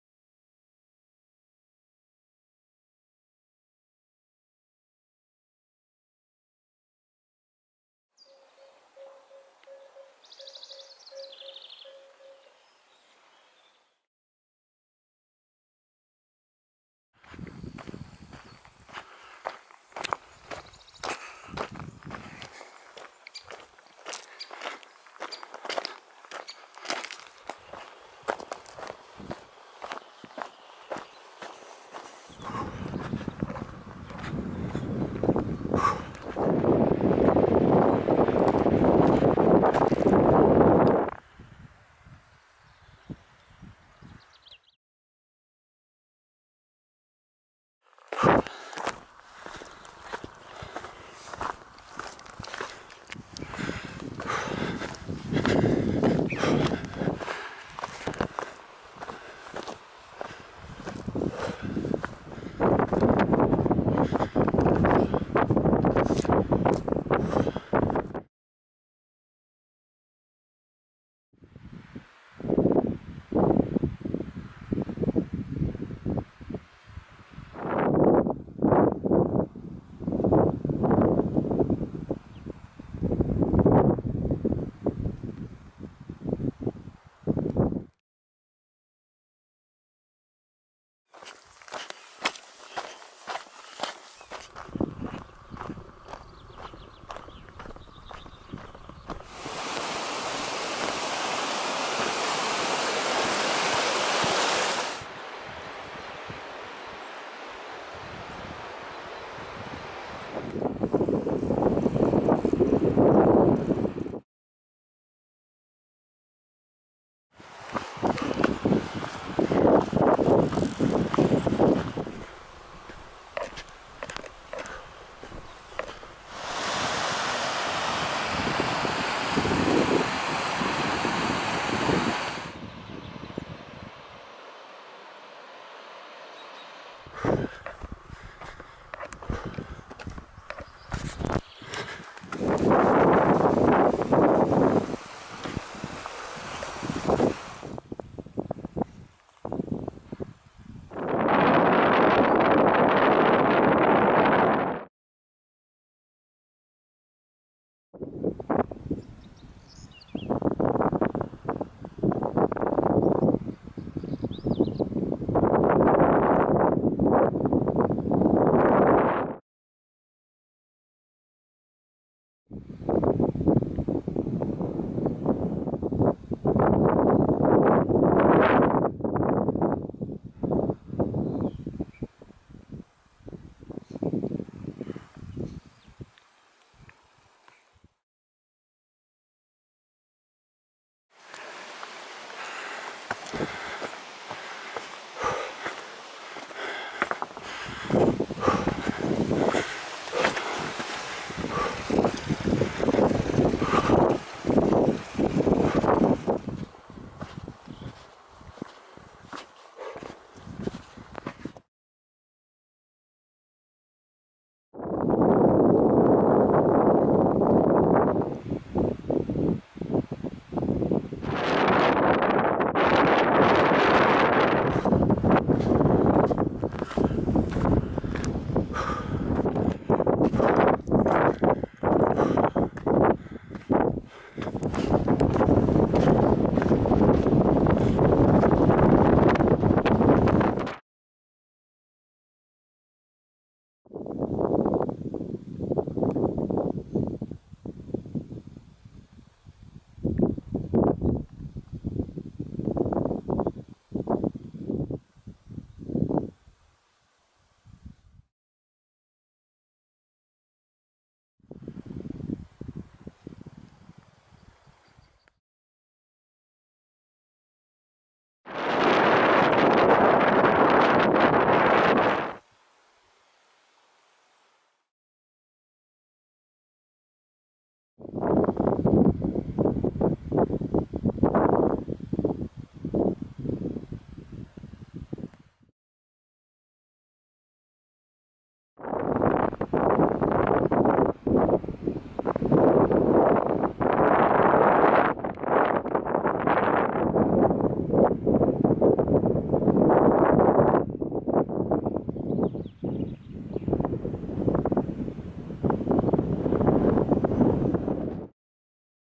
En remontant les Merveilles - Huit jours en juillet dans le Mercantour - Jour 4
Une randonnée en boucle depuis Castérino, par la Baisse de Fontanalbe, la Vallée des Merveilles, la Baisse de Valmasque et les Lacs du Basto, Noir et Vert.